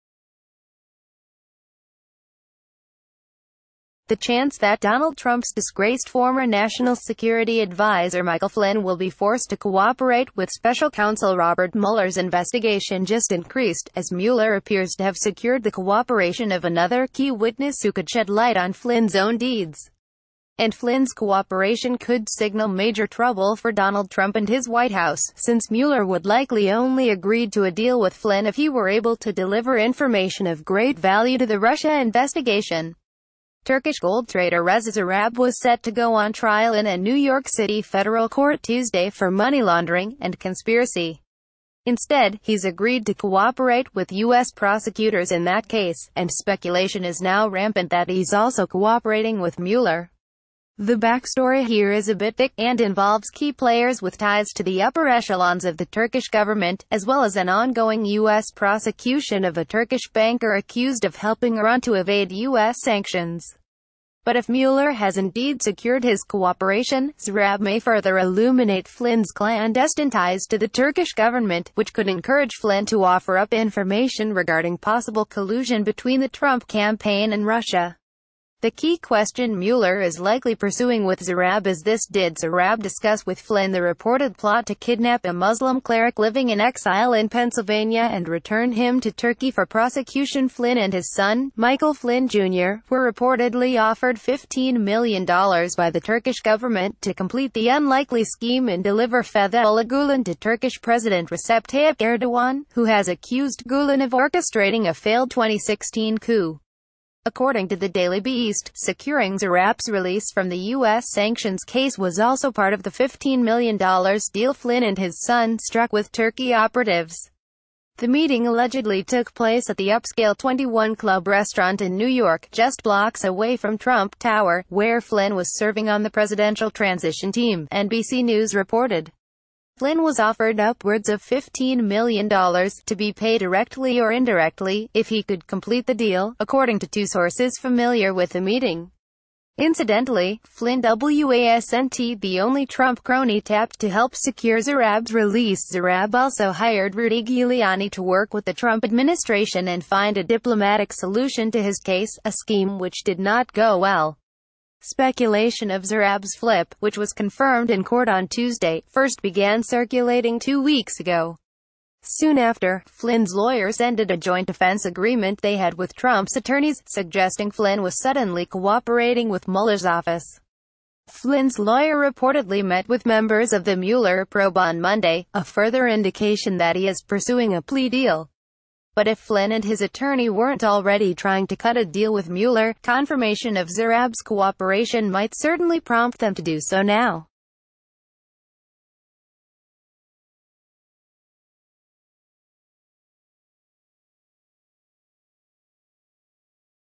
The Special Advisor may have just flipped a vital witness in the probe of Russia | Trump breaking n
The Special Advisor may have just flipped a vital witness in the probe of Russia | Trump breaking news
The chance that Donald Trump’s disgraced former national security adviser Michael Flynn will be forced to cooperate with special counsel Robert Mueller’s investigation just increased, as Mueller appea...